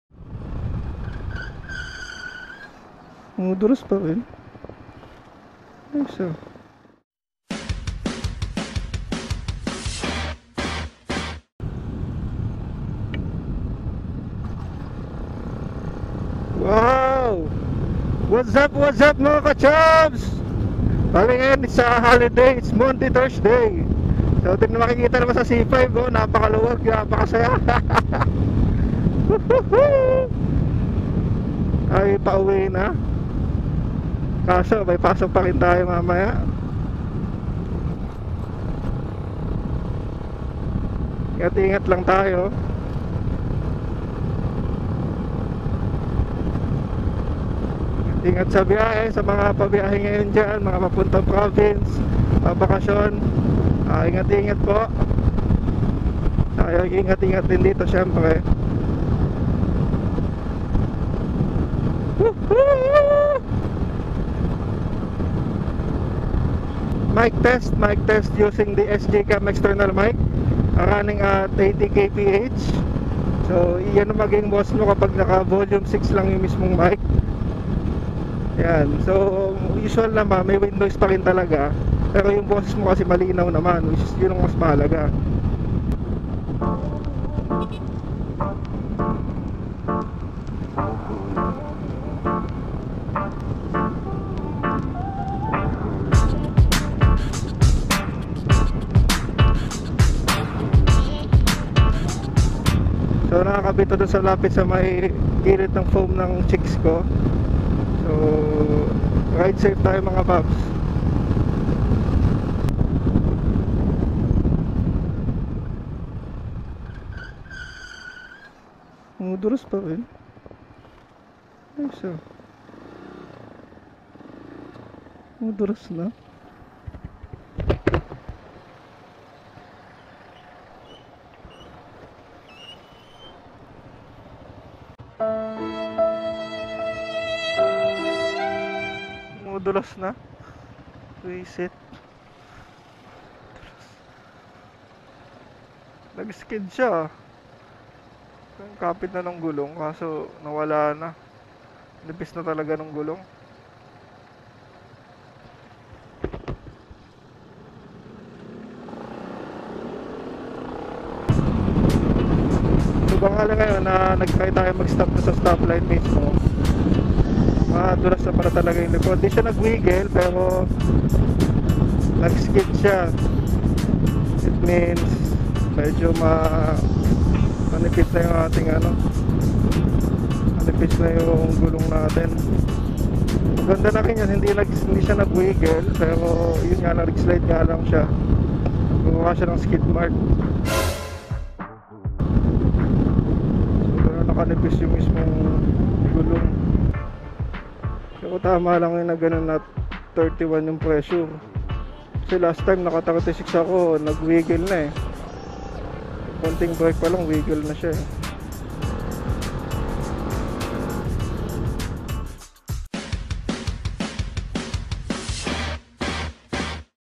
SJ6 External Mic Test and Shit Happens | Chubs Motovlog
Chub Chub Chubby Rides!

Watch my vlogs and adventures! Eto yung time na may nangyareng hinde maganda habang ongoing yung mic test.